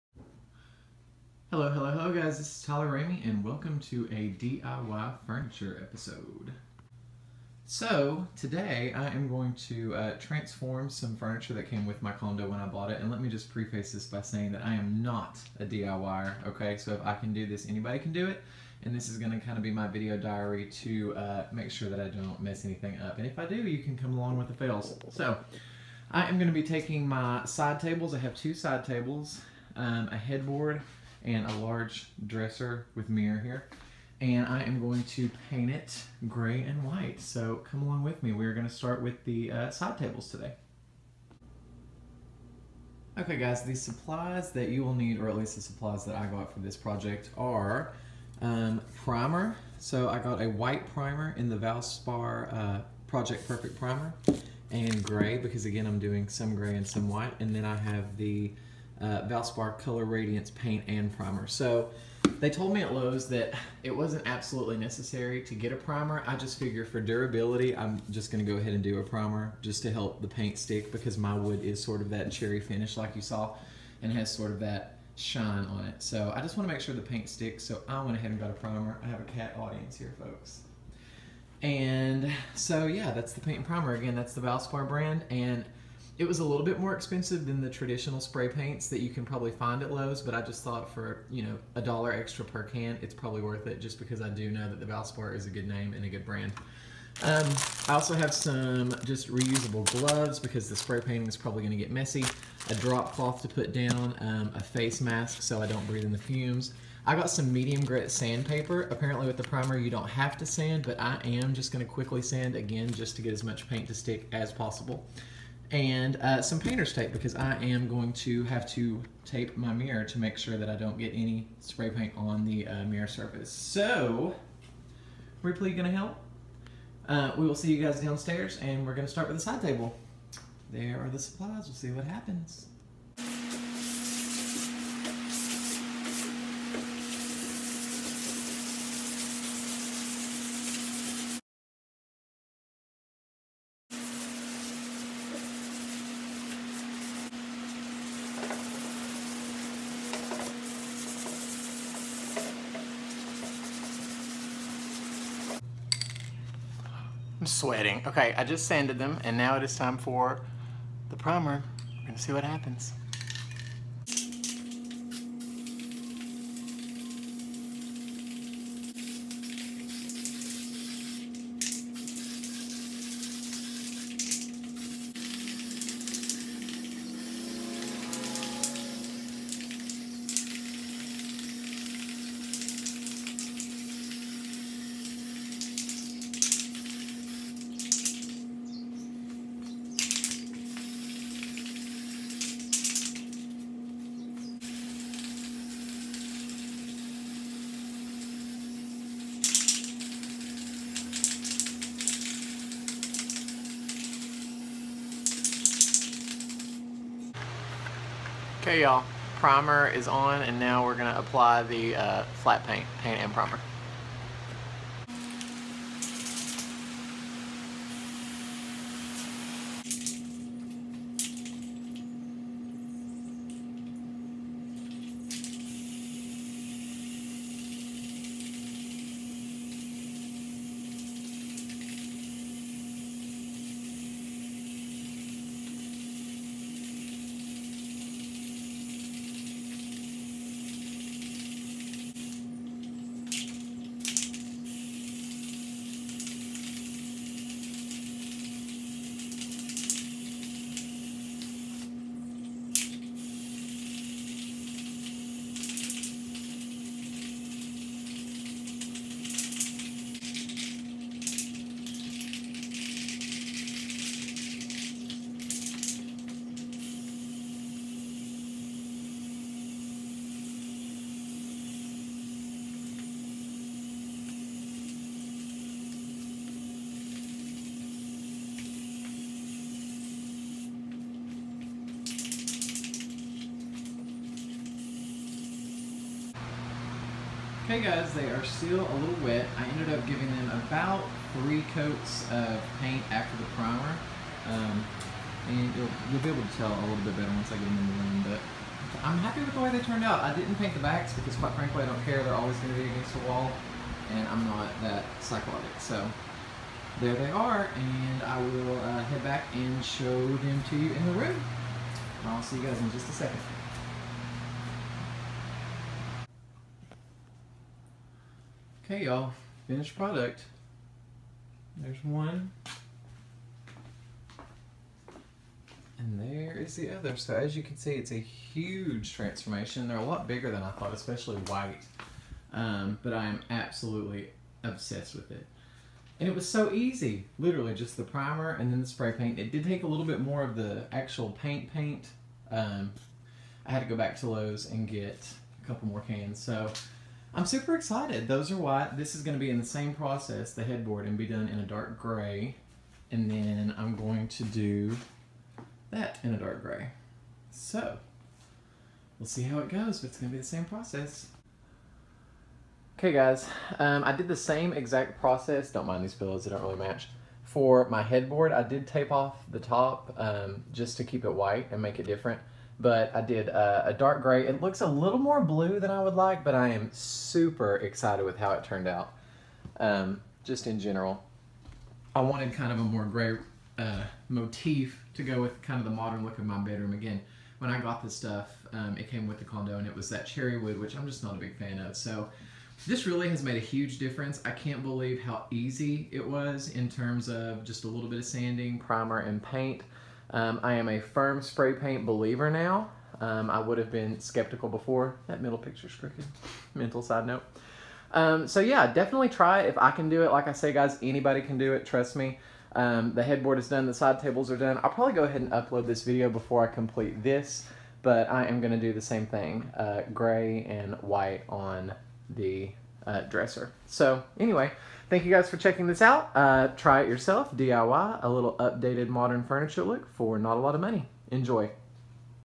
DIY Spray Painted Furniture 🛋🛏💖
Join me as the most NON-DIY person takes cherry wood bedroom furniture and gives it a spray painted gray and white touch.

This was super easy, and I am ecstatic how it turned out.  Trust me that when I say that if I can do it...anyone can.

What you will need:
-Spray Paint Primer (I used Valspar Project Perfect) ((this is apparently optional, but I wanted to cut down on coats of the actual paint))
-Spray Paint & Primer (same brand.  Flat White and Silver Fox)
-Medium grade sand paper
-Gloves (save those hands!)
-Mask (the fumes!!! Shew)
-Dropcloth (save your floor!)

And be sure to do this in a well ventilated spot.  Garage our outside preferably.

I did sand and prime both of which I had heard was optional.  In my opinion though...just a light sanding made all the difference in the paint sticking.  I waited about 15 minutes between coats.  Did 3 coats of paint on the side tables (white) and 2 coats on the headboard (gray)

I typically do "Mukbang" or eating videos.  Try it, you may like it!  Join me for a meal and some crazy conversation.